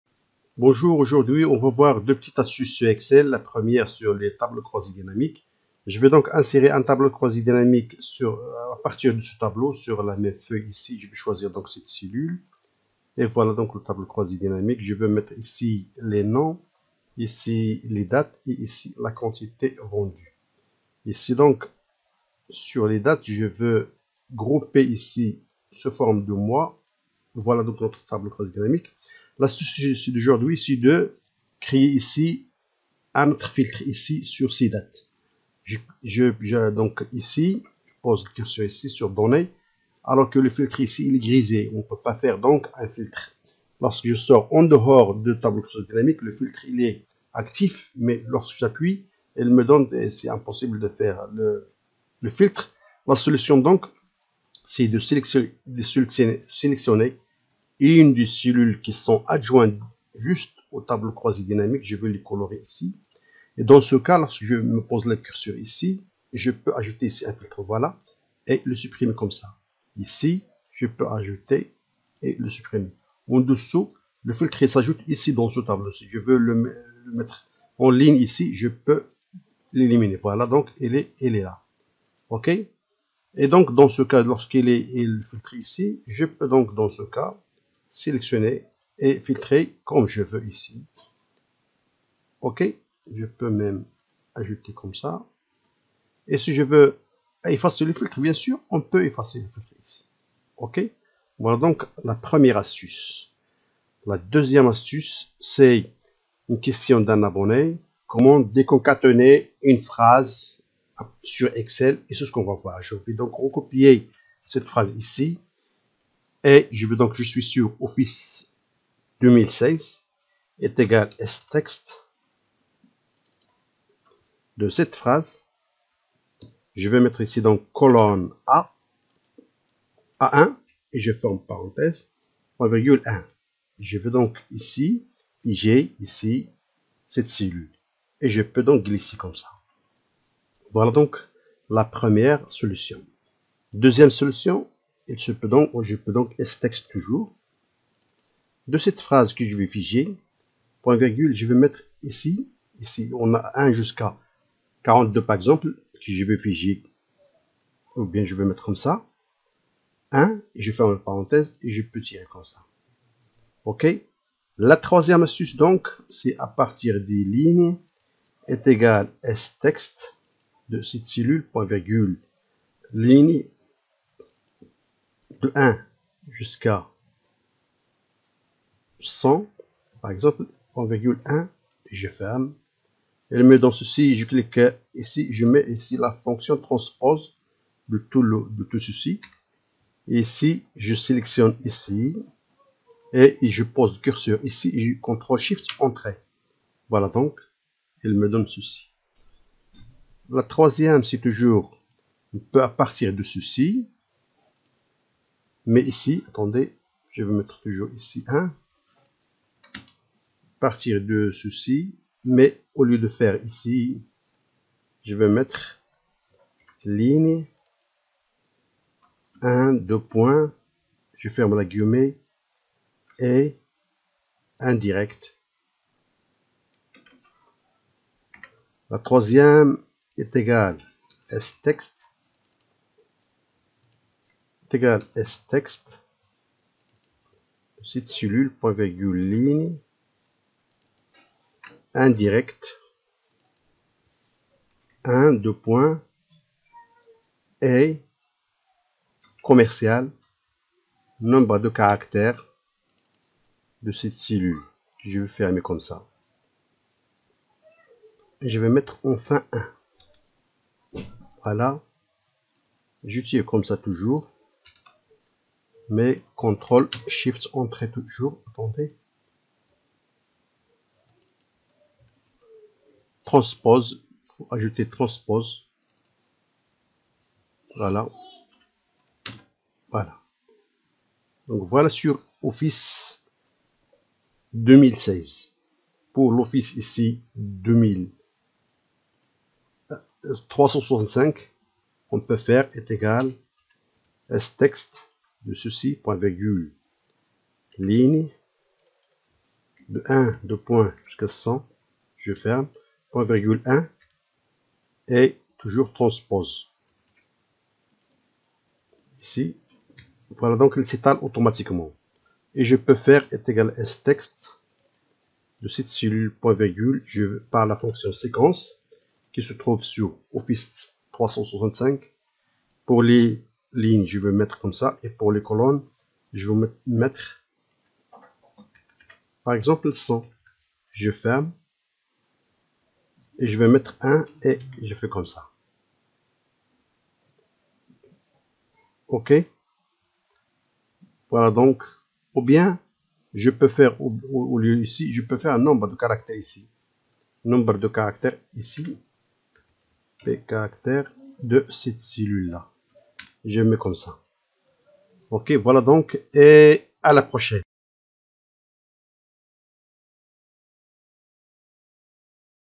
2 Astuces Excel Tableau crois dynamique &decortiquer ph en lettre
2 astuces sur excel
1-sur les tableaux croisés dynamique
2-comment deconcatenerer une phrase en lettre

astuce 2 commentastuce 2ici   1:45